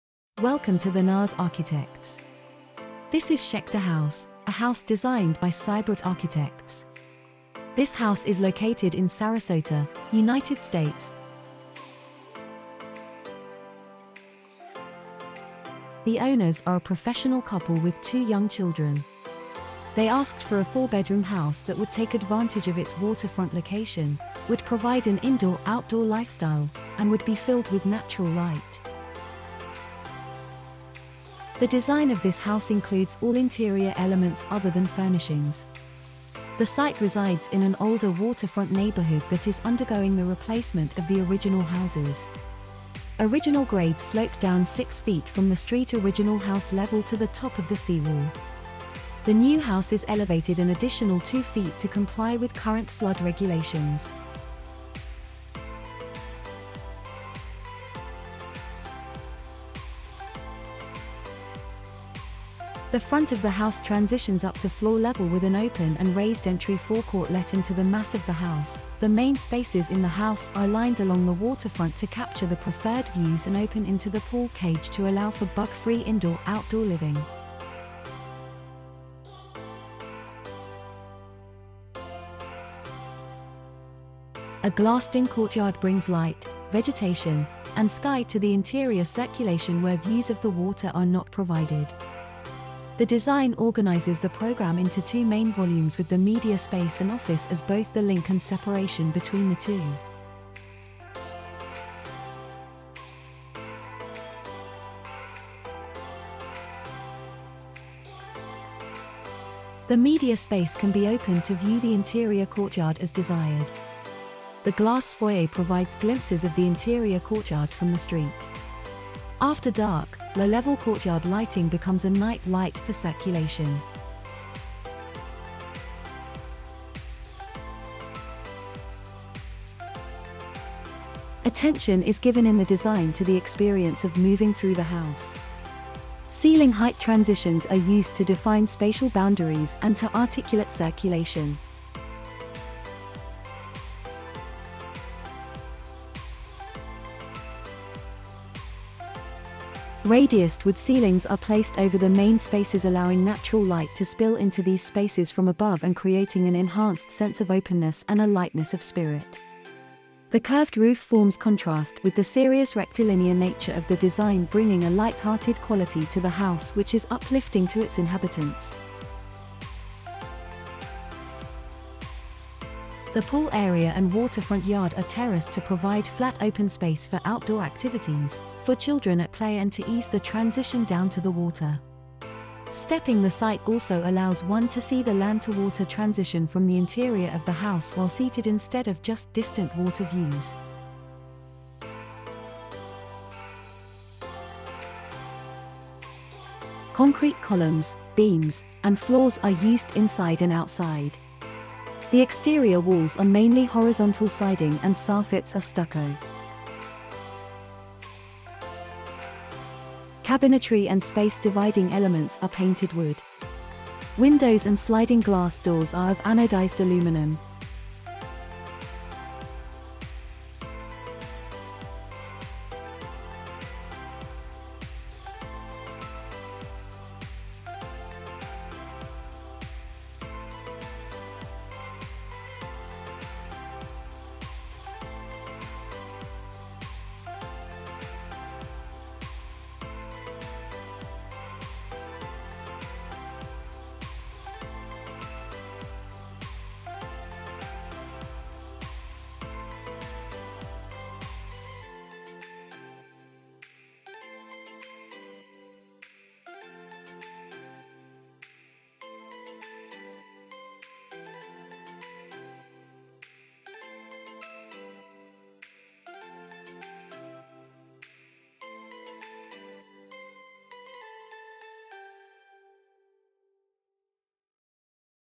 Schecter House / Seibert Architects (Sarasota, United States) - Architects, Design, Plan & Ideas
Architects: Seibert Architects
Area: 4140 ft²
Photographs: Ryan Gamma Photography
Manufacturers: James Hardie, Omnia Industries, COOPER/HALO LIGHTING
Lead Architects: Samuel C. Holladay, Michael Lee Epstein